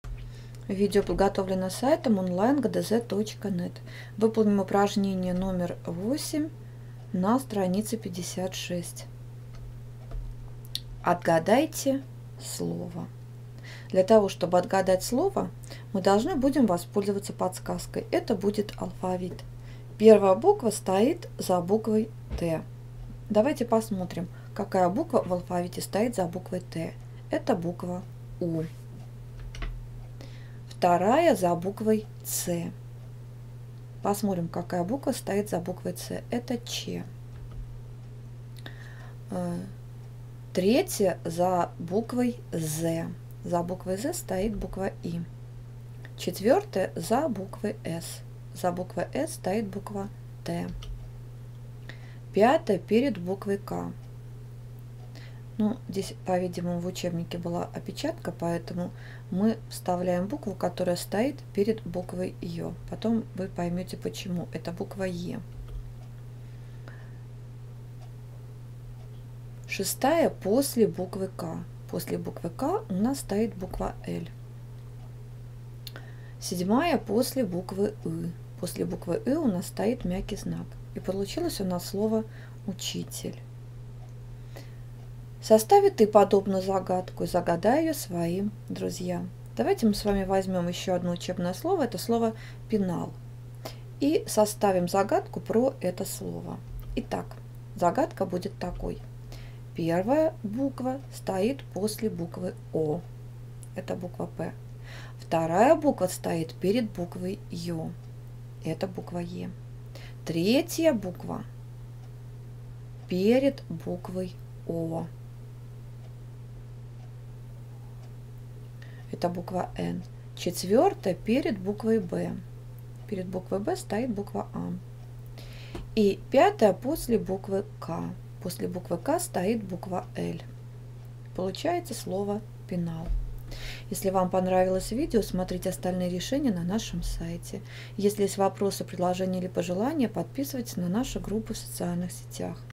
Страница 56 Упражнение 8 «Русский алфавит, или Азбука» - Русский язык 1 класс (Канакина, Горецкий)
Чтобы выполнить упражнения №8 на странице 56 необходимо повторить алфавит и уметь определять буквы, которые стоят перед или после данной. Без труда с этим справиться поможет наше видео.